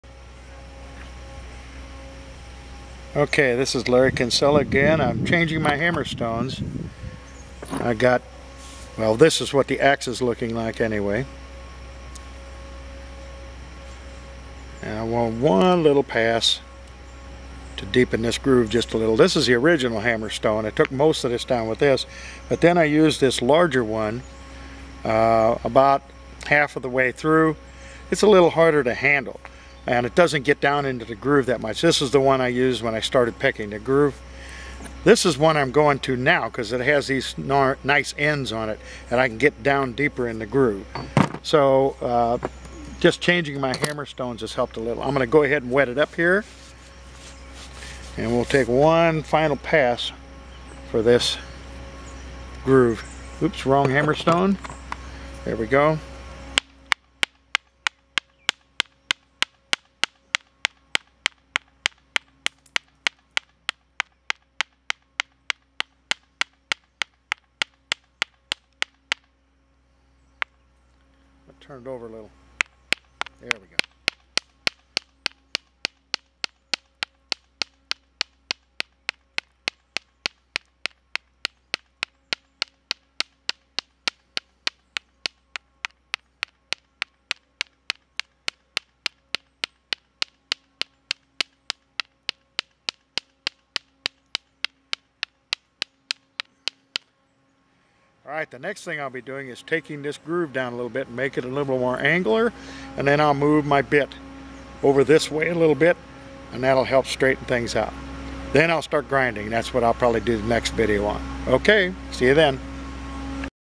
Pecking the groove
Larry finishes pecking the groove, in the ax, using the chert hammerstone.